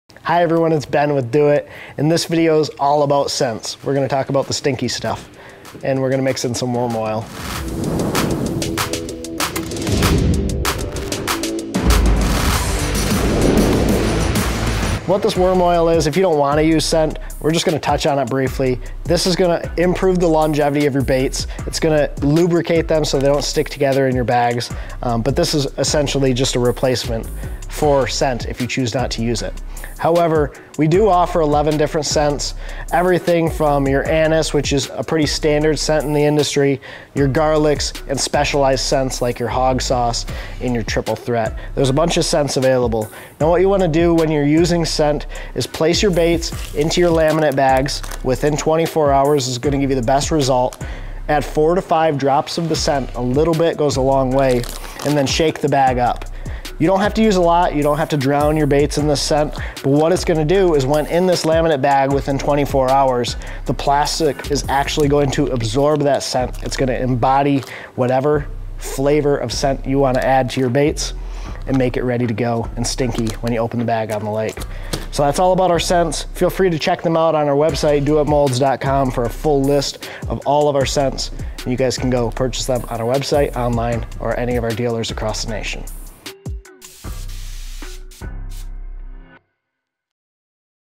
Adding Scents to Your Baits
In this How We Do-it Video, Benjamin Nowak takes you through the process of adding scents to your custom soft plastic baits.